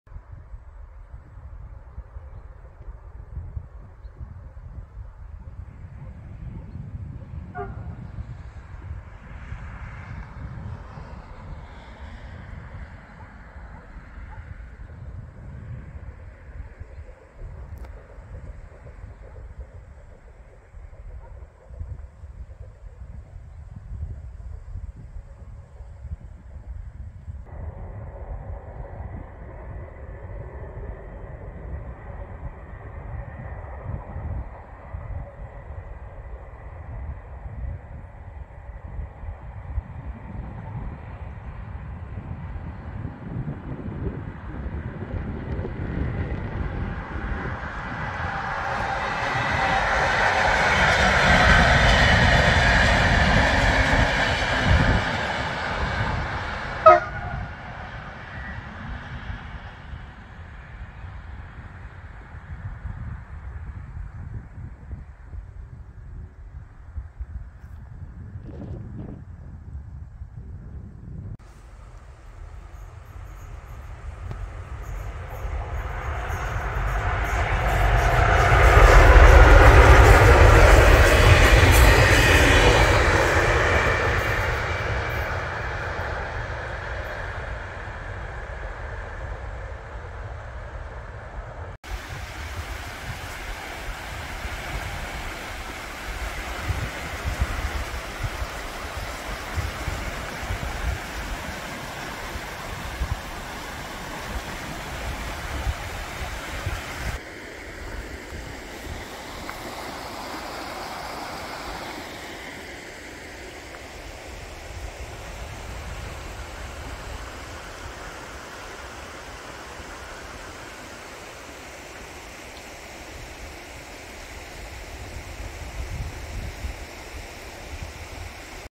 Бърз влак БДЖ N 3623,на фона на Стара Планина,до карловското село Куртово.
Чува се от стрелките на прелеза след гара Ботев.  Пътническият влак е Зимница-Карлово. Моста е над Бяла река.